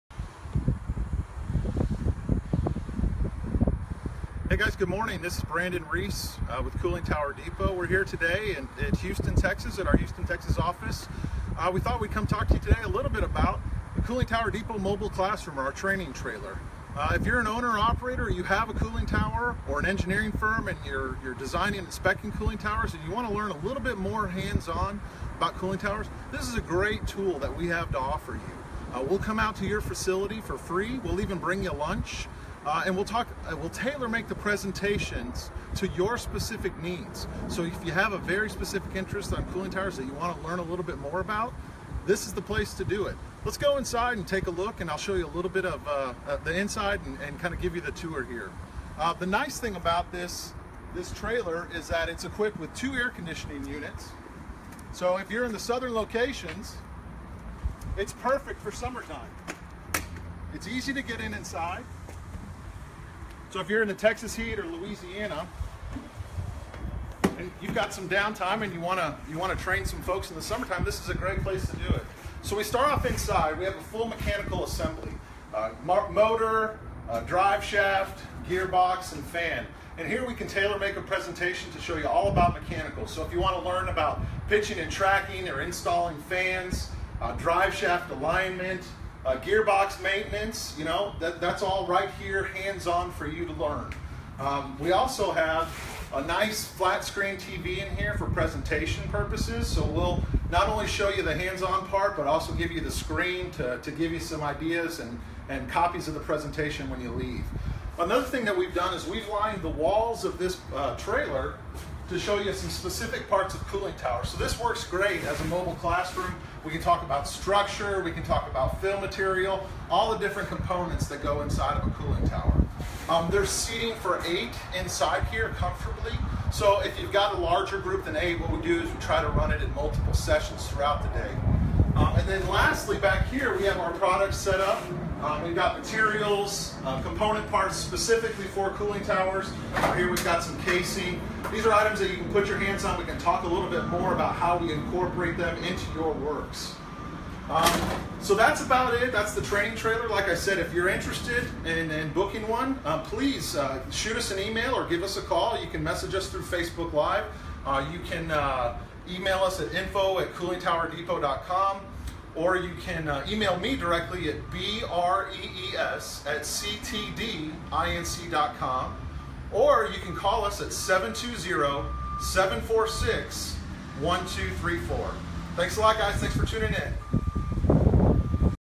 Cooling Tower Depot's Mobile Classroom
Brandon Rees gives us a look inside our intovative CTD Mobile Classroom. We come to you, bring you lunch, and show you proper cooling tower procedures and helpful tips.